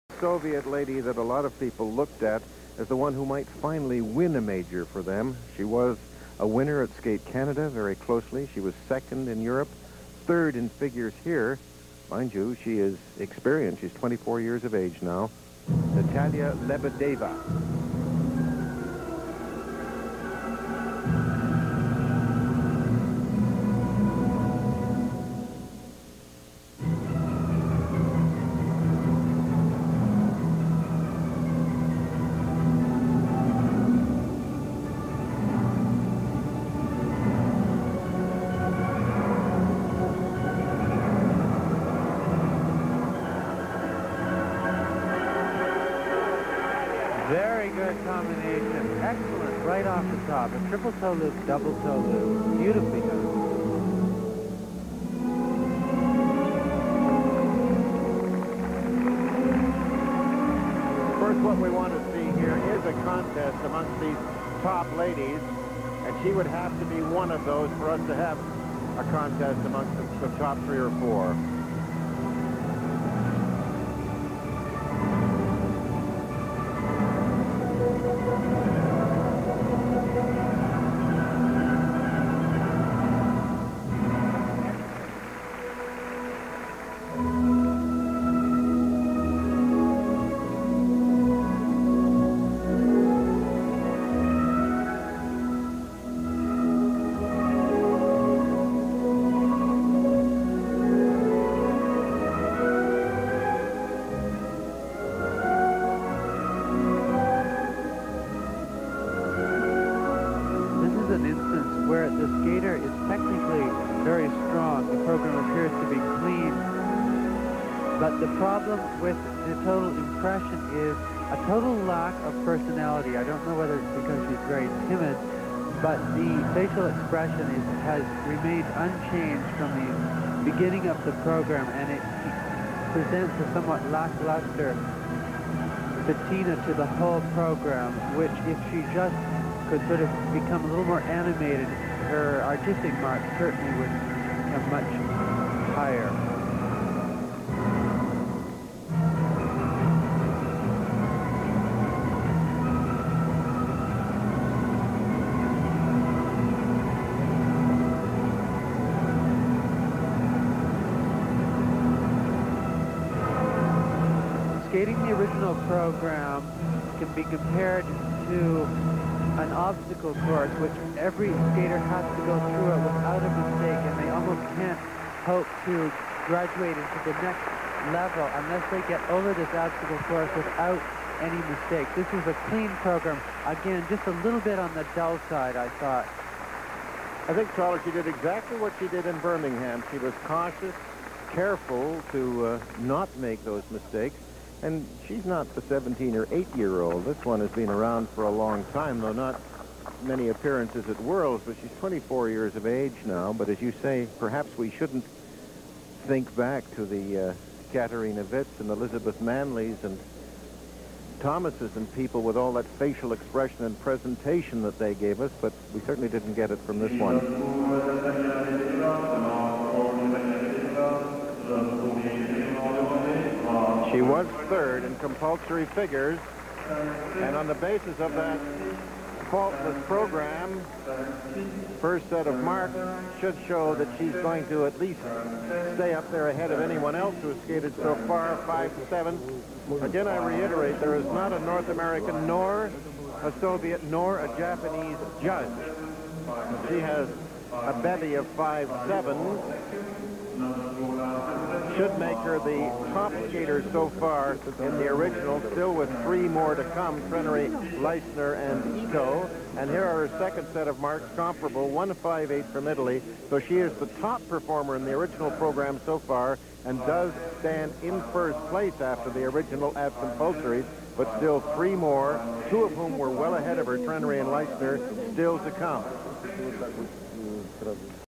Natalia Lebedeva - 1989 World Championships Short Program
Natalia Lebedeva was sitting in 3rd place after compulsory figures at the 1989 World Championships (her 4th appearance).  This short program placed 4th in the segment, and she was in 4th place overall heading into the long program.